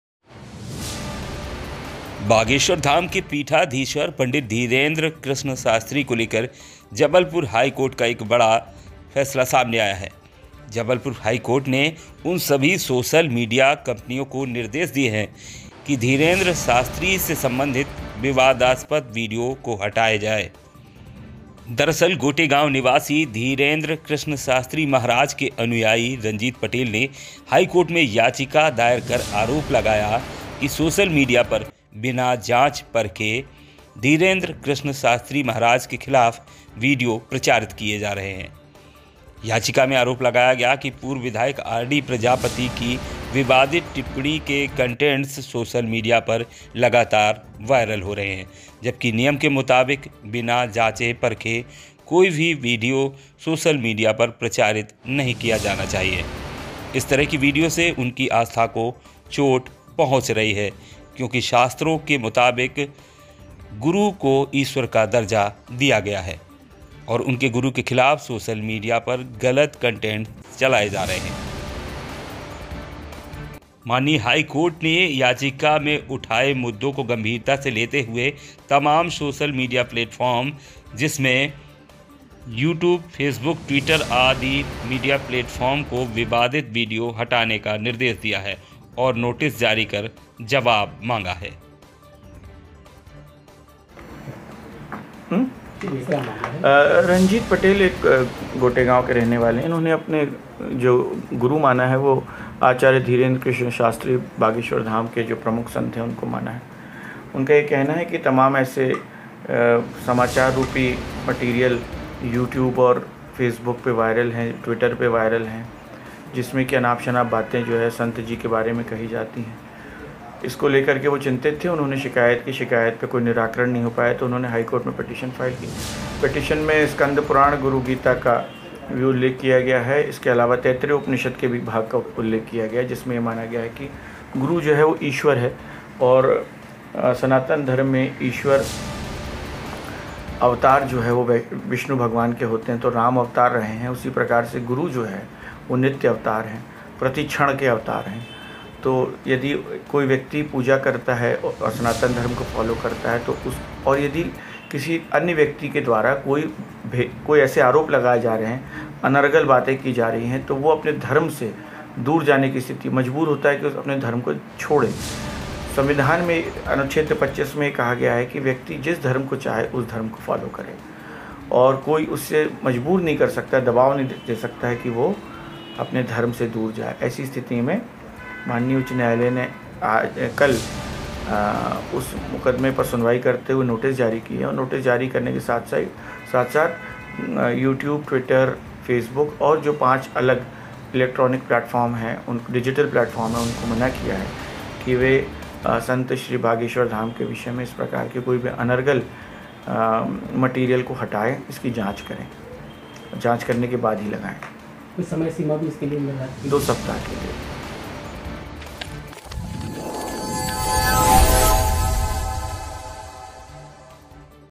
हाईकोर्ट के निर्देश,सोशल मीडिया से हटाए जाएं बागेश्वरधाम के धीरेंद्रमहराज को लेकर विवादास्पद वीडियो
हाईकोर्ट ने दिए निर्देश,सोशल मीडिया प्लेटफार्म्स से हटाए जाएं बागेश्वरधाम के पीठाधीश्वर धीरेंद्र कृष्ण महराज को लेकर विवादास्पद वीडियो